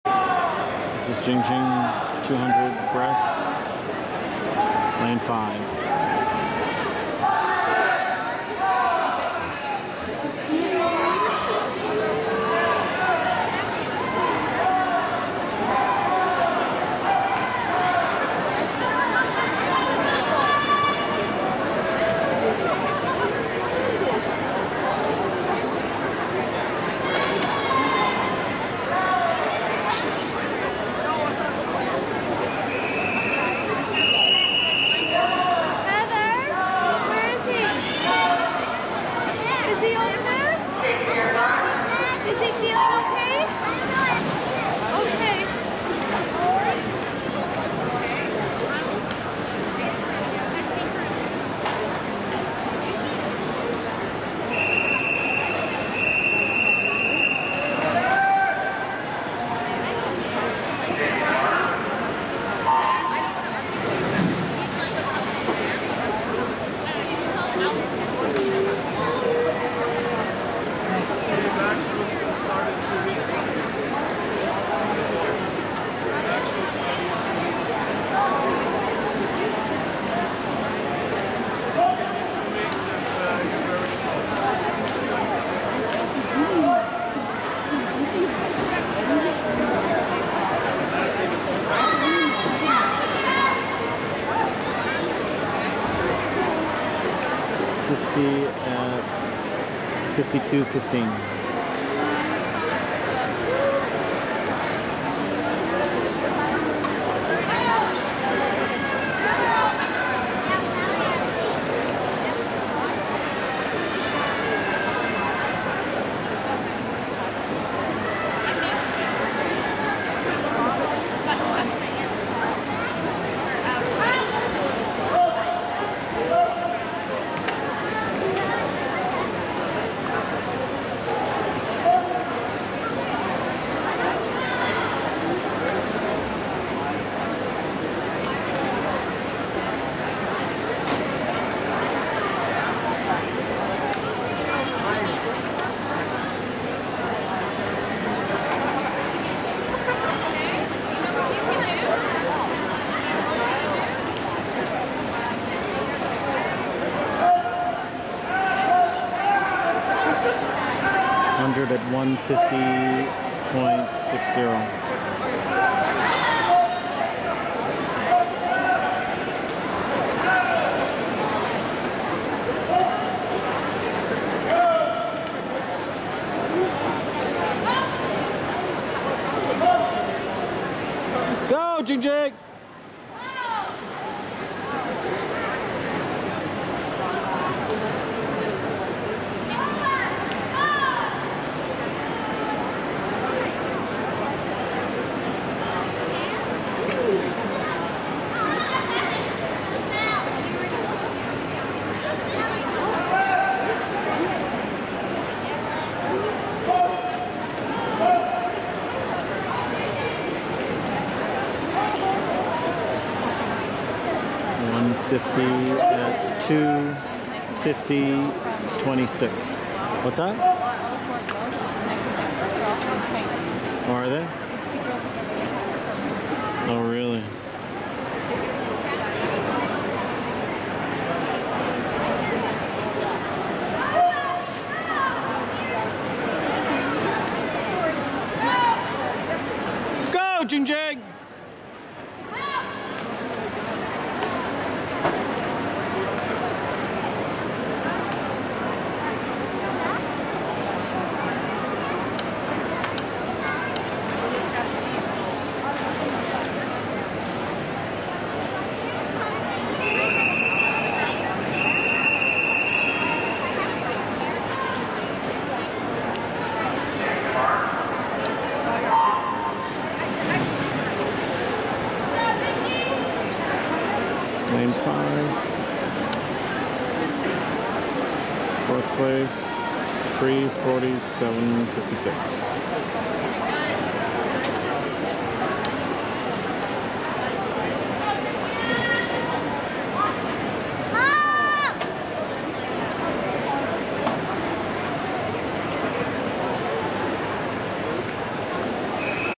2015/01/11 JingJing 200M Breast 11&O Girls Keyano Blue Bears E39H2
2015 Jan 11 09:18am
JingJing Wu
200M Breaststroke 11&U
Girls
Event 39
Heat 2
Lane 5
Official Time - 3:47.56s

splits
50M - 52.15s
100M - 58.45s
150M - 59.66s
200M - 57.30s

Heat Placed - 4th
Final Placed - 15th

Madisson Young - Lane 1
Skylar Sabasch - Lane 2
Abby Oliver - Lane 3
Kyra McMurray - Lane 4
JingJing Wu - Lane 5
Ashley Fehler - Lane 6
Paulina Teske - Lane 7
Jadyn Bunbury - Lane 8